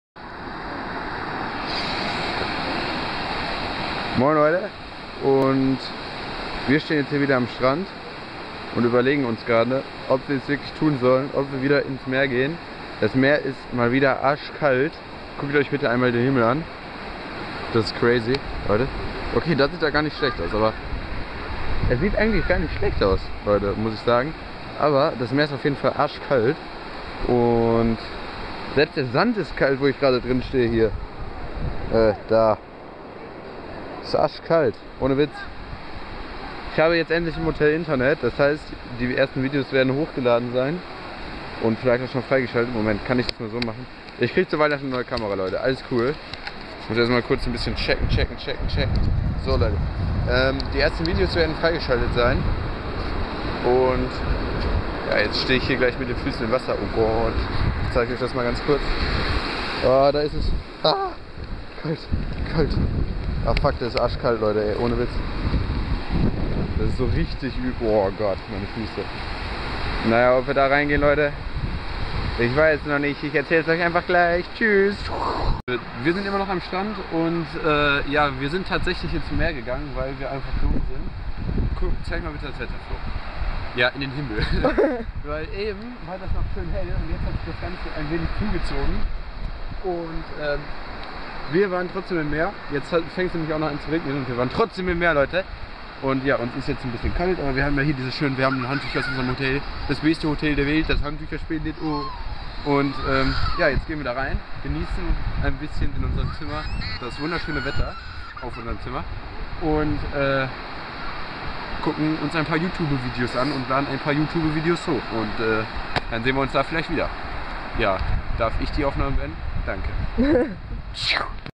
NORDERNEY VLOG Tag 4 - Strandchiller [Deutsch] [Full/HD]
Den Tag wieder nur am Strand von Norderney bei scheiß Wetter verbracht.
Wir sind halt ziemlich dumm! :D